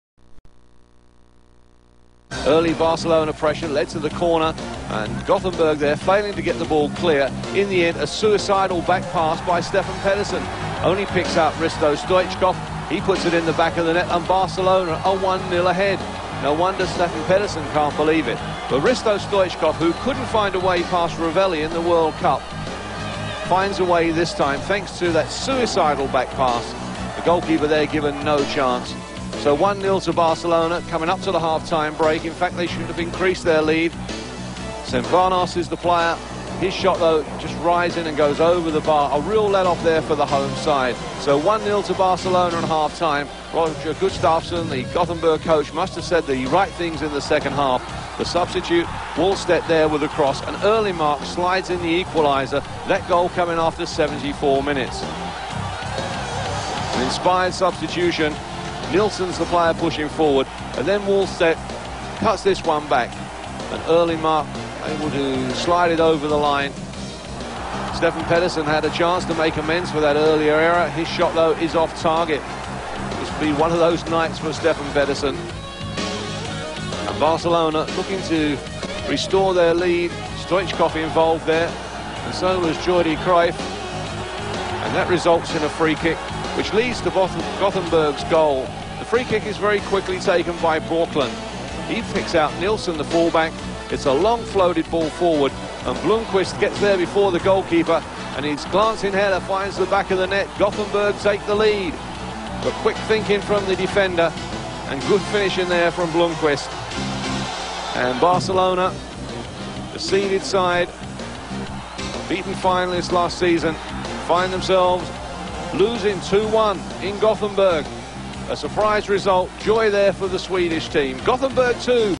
IFK Goteborg vs Barcelona (UEFA Champions League 1994/1995)
UEFA Champions League 1994/1995 (Group A)
28 September 1994, Ullevi, Gothenburg (32,215)
Referee: Bernd Heynemann (Germany)

                   IFK Goteborg  2-1  Barcelona
                 Erlingmark 74'          Stoichkov 10'
                  Blomqvist 89'